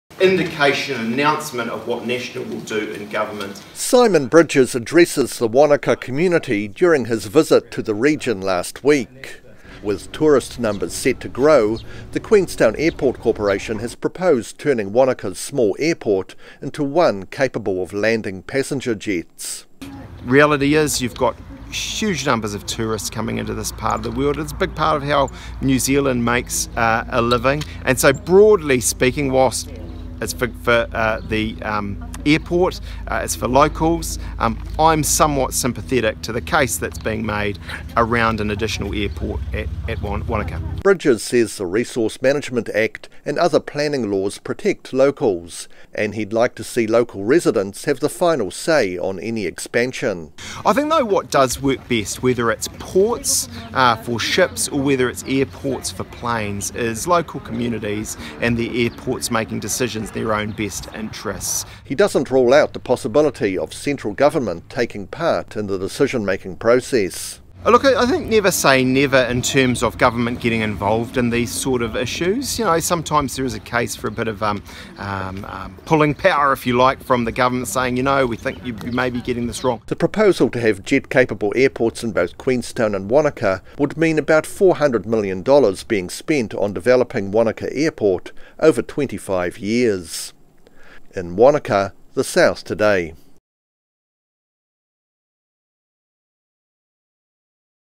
National not standing in way of airport expansion
The National Party is unlikely to stand in the way of expanding Wanaka airport, if elected next year.